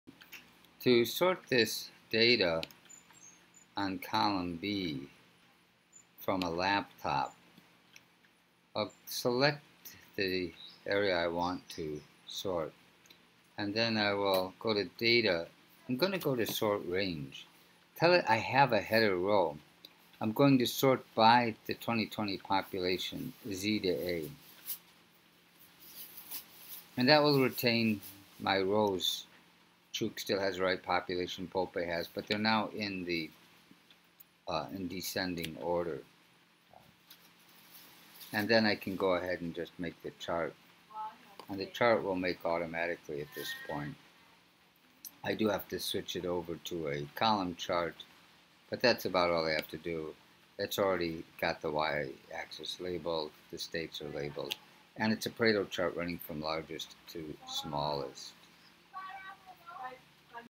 Stats 3.1 Pareto chart homework on a computer
How to complete the Pareto chart homework from section 3.1 on a laptop or desktop computer